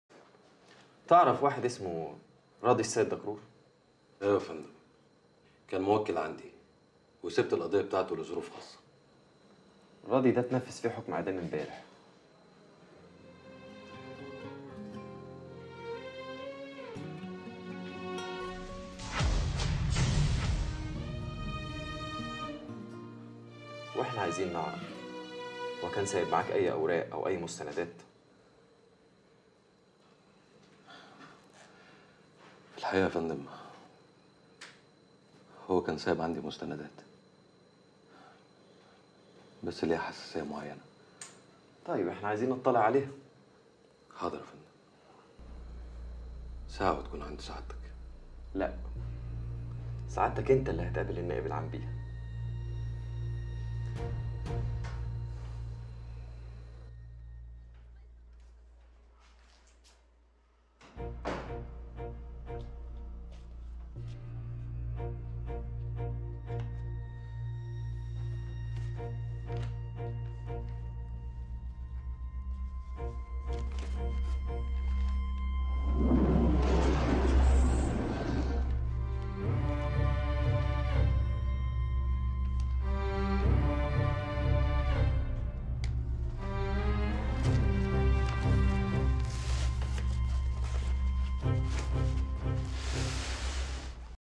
أحمد_منير معاه مستندات وهيقدمها للنائب العام
معاه مستندات ورئيس النيابه هيخليه يقدمها للنائب العام